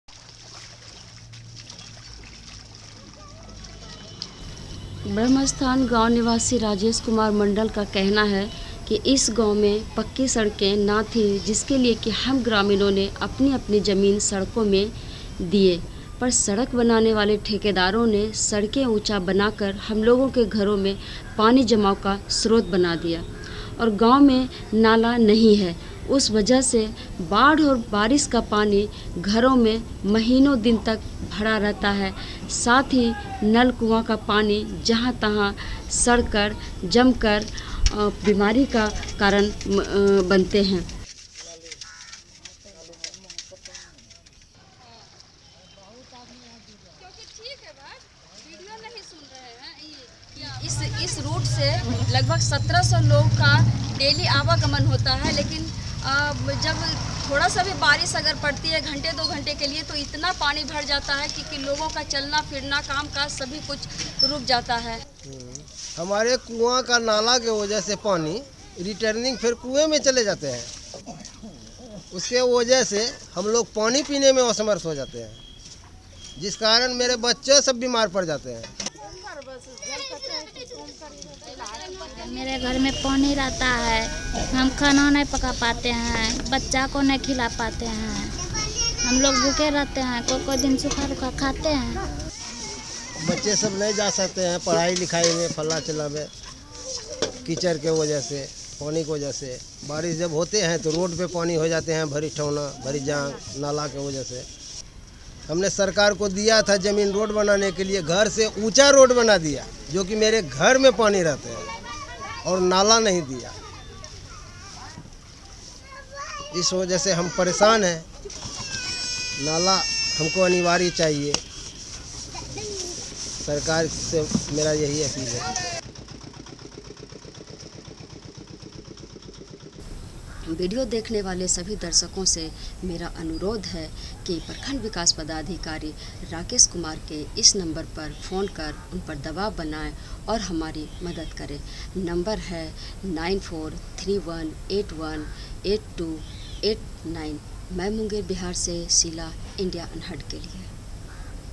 Waterlogging In Houses In Bariarpur Village, Munger
Community Correspondent Sheela Kumari reports from Munger, Bihar for Video Volunteers.